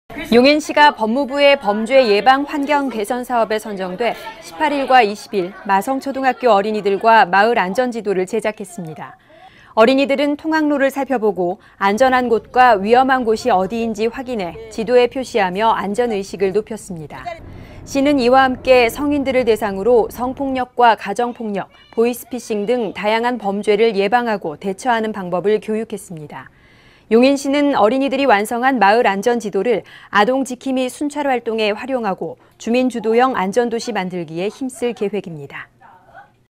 [용인시정뉴스] 아이들 손으로 만드는 마을안전지도
용인시가 법무부의 범죄예방 환경개선사업에 선정돼, 18일과 20일 마성초등학교 어린이들과 마을안전지도를 제작했습니다. 어린이들은 통학로를 살펴보고 안전한 곳과 위험한 곳이 어디인지 확인해 지도에 표시하며 안전의식을 높였습니다.
시는 이와 함께 성인들을 대상으로 성폭력과 가정폭력, 보이스피싱 등 다양한 범죄를 예방하고 대처하는 방법을 교육했습니다. 용인시는 어린이들이 완성한 마을안전지도를 아동지킴이 순찰활동에 활용하고, 주민주도형 안전도시 만들기에 힘쓸 계획입니다.